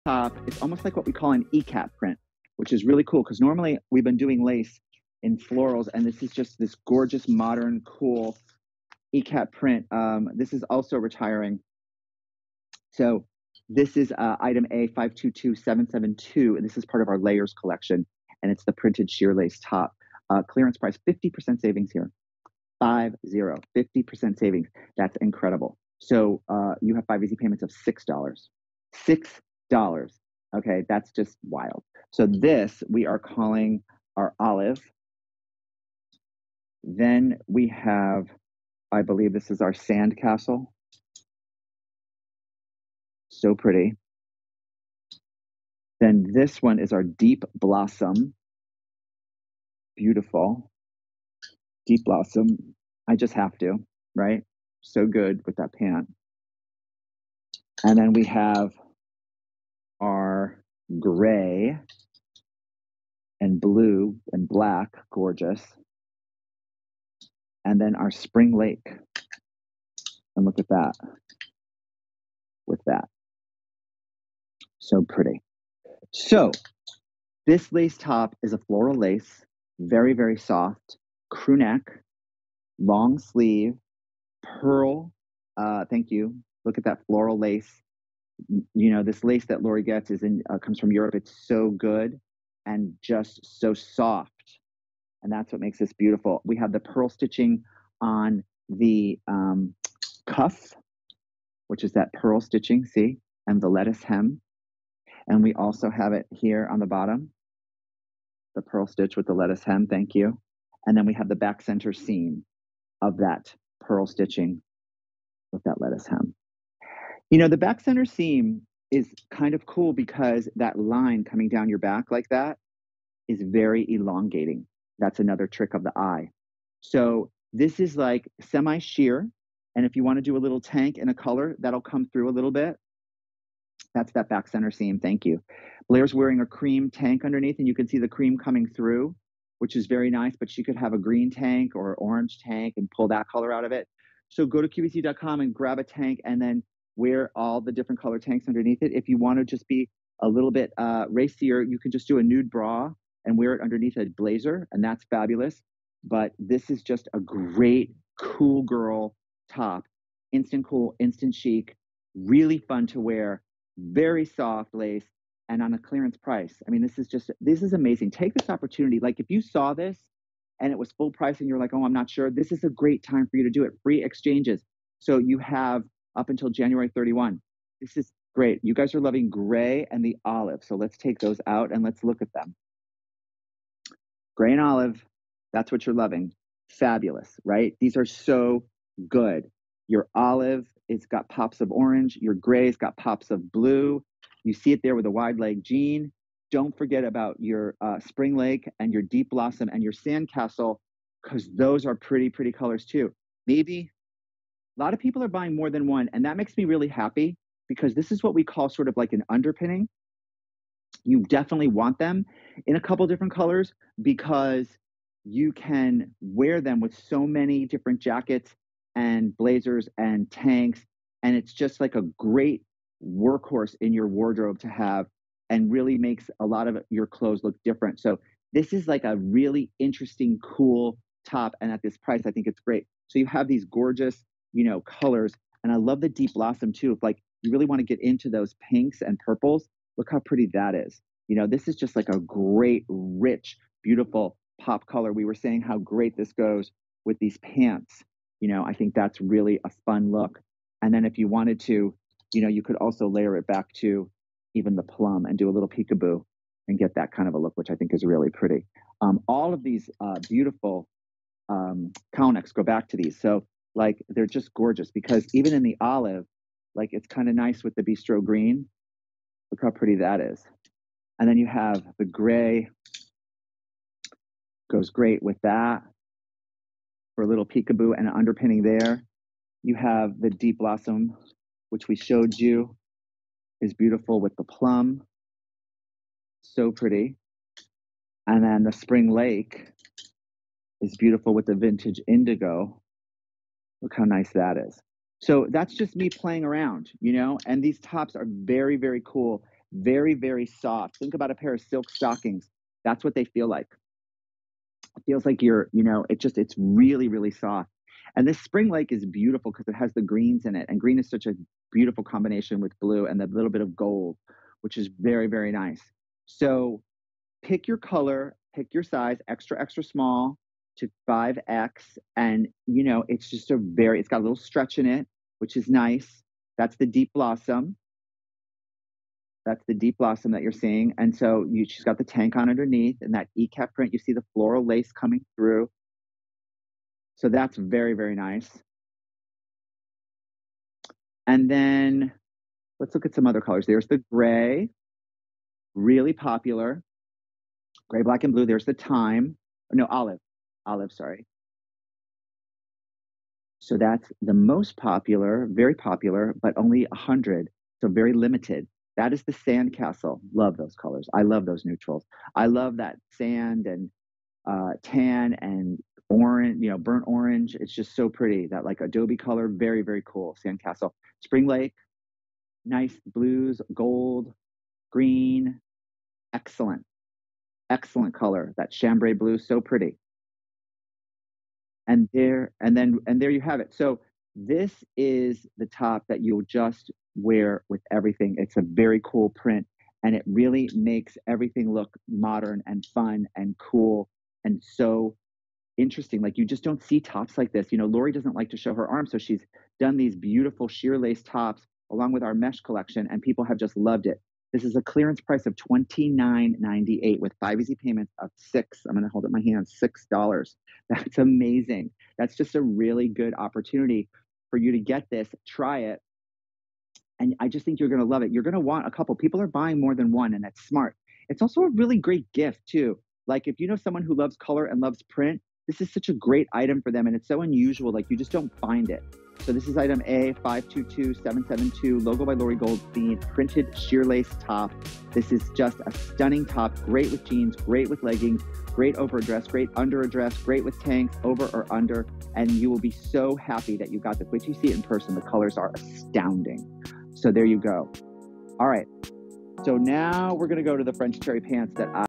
LOGO Layers by Lori Goldstein Printed Sheer Lace Top on QVC
LOGO Layers by Lori Goldstein Printed Sheer Lace Top

That's the way we like it! A ladylike lace fabrication and contrasting lettuce stitch bring sweet and sassy vibes to this pretty printed top. From LOGO by Lori Goldstein&reg;.